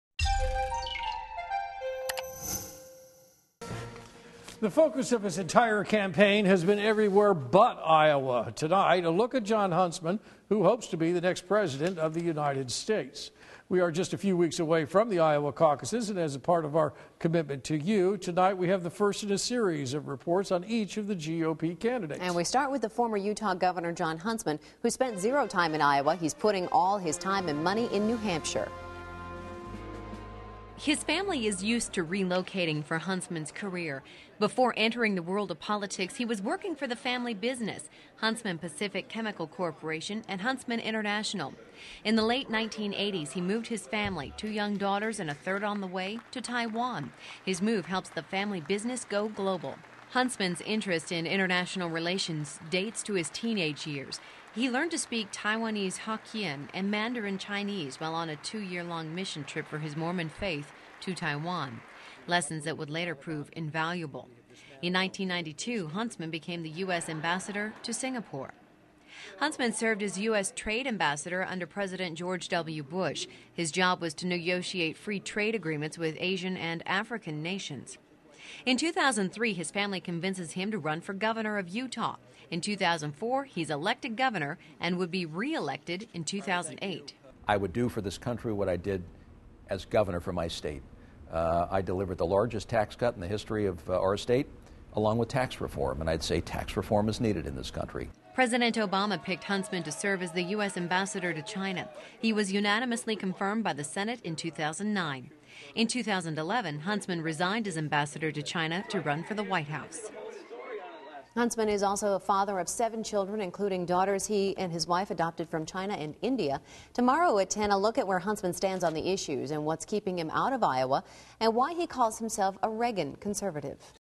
Candidate Profile: John Huntsman
Former Utah Gov. Jon Huntsman has spent little time in Iowa and is putting all his time and money in New Hampshire.